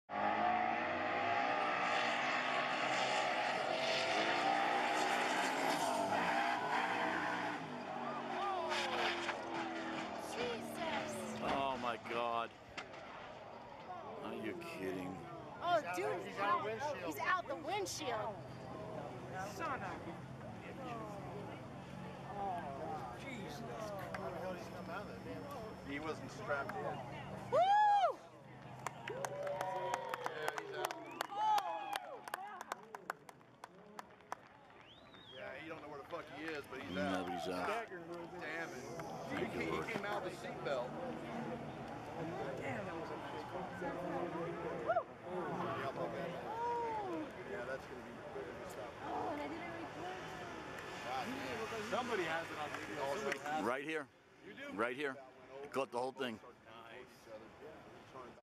حادث بسيارة سباق يخرج السائق من الزجاج الأمامي
ألقي بالمتسابق، كيلي هارفي، من الزجاج الأمامي لسيارته الكلاسيكية خلال سباق ليخرج منها وهو يمشي.